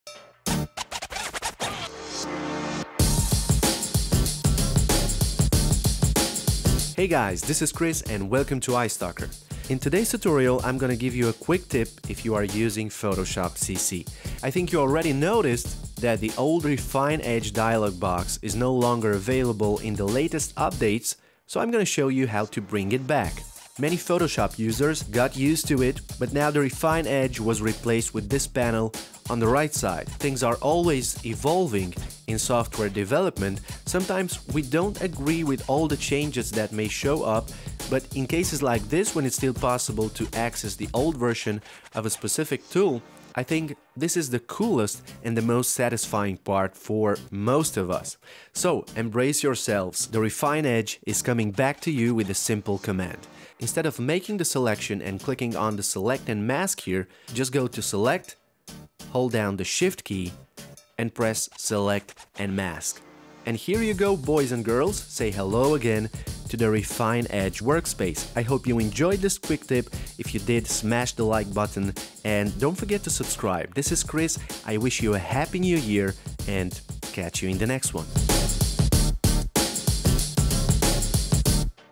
Where to find the OLD Refine Edge Tool: Photoshop Tutorial
👉 Best Music! Free 30 days Trial For YouTube Creators

I think you already noticed that the old refine edge is missing in the latest updates so in this Photoshop tutorial I’m gonna show you how to find it and bring it back with a simple shortcut.
Many Photoshop users got used to it, but now the refine edge is gone and it was replaced with a panel on the right side. Things are always evolving in software development, sometimes we don’t agree with all the changes that may show up, but in cases like this, when it’s still possible to access the old version of a specific tool, I think this is the coolest and the most satisfying part for most of us.
So if you can't find the Refine Edge tool, this video is for you.